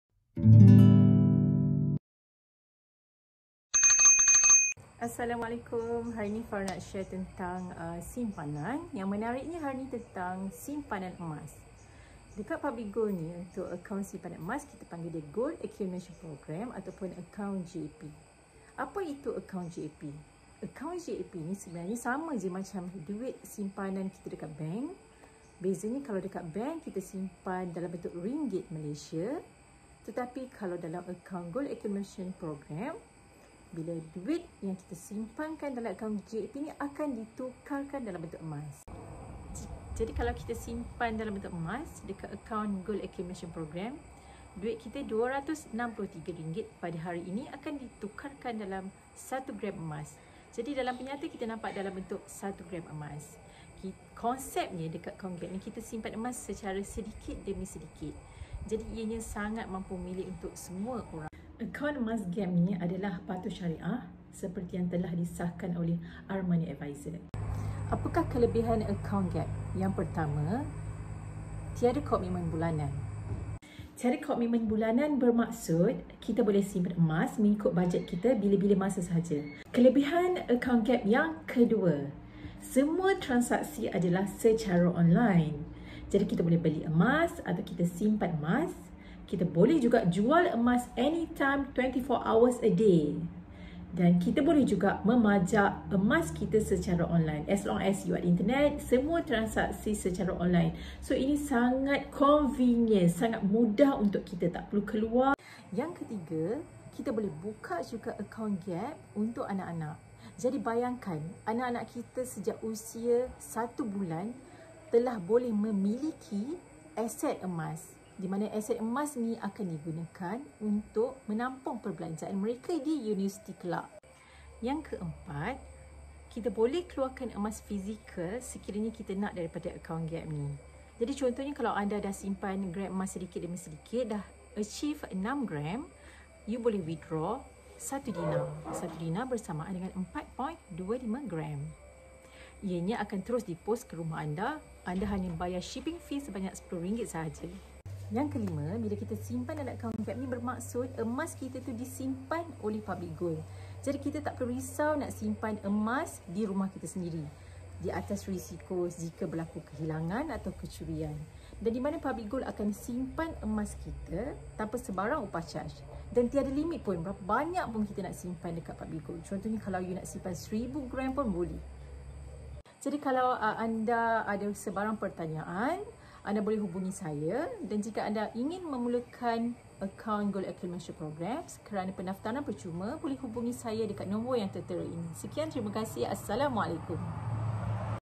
5 kelebihan akaun simpanan emas GAP ( Gold Accumulation Program)
[KELEBIHAN AKAUN GAP]
📌 Kita boleh beli emas dengan budget sekecil RM100 (0.3xxgm) atau 1 gram sahaja.
📌 Simpan emas dengan PG secara percuma, tanpa had masa dan kuantiti
📌 Boleh keluarkan emas fizikal gold bar atau dinar dari mana-mana 18 cawangan PG seluruh Malaysia.
📌Boleh beli dan jual secara online tanpa perlu ke cawangan Public Gold iaitu secara online shj.
📌Dapat beli emas 1 gram dengan harga paling murah di Malaysia.
📌 Tiada yuran simpanan (disimpan secara percuma).
📌Disokong 100% oleh emas fizikal.
📌 Pegangan baki minima di dalam akaun 1 gram sahaja.
📌 Urusniaga bila-bila masa. 24 jam sehari, 365 hari setahun.
📌 Anda boleh mengeluarkan item emas fizikal serendah 1 gram.
📌 Akaun GAP tiada tarikh luput
📌 Harga ketulenan 999.9 per gram yang jauh lebih murah berbanding kedai emas
📌 Boleh buka akaun emas untuk anak dari seawal lahir.